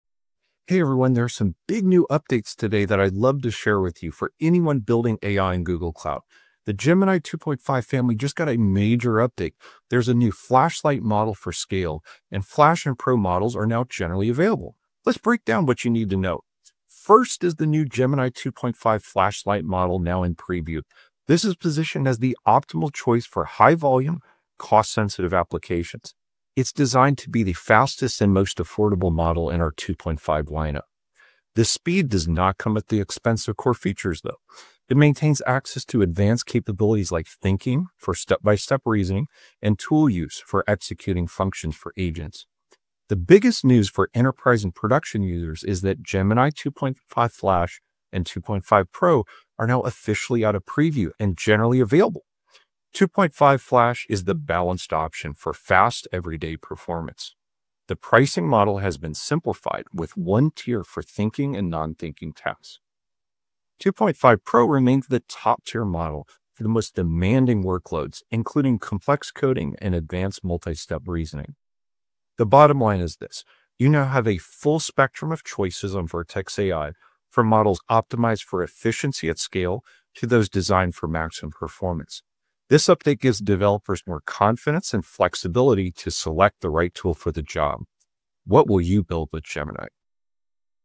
Gemini 2.5 - What you need to know
Big news for developers building with AI on Google Cloud:

🔹 Gemini 2.5 Flash & Pro are now Generally Available. This is the green light for moving from preview to production with confidence, stability, and support.

🔹 Gemini 2.5 Flash Lite is now in Preview. It’s engineered as the fastest and most cost-effective 2.5 model, targeting high-volume, low-latency tasks.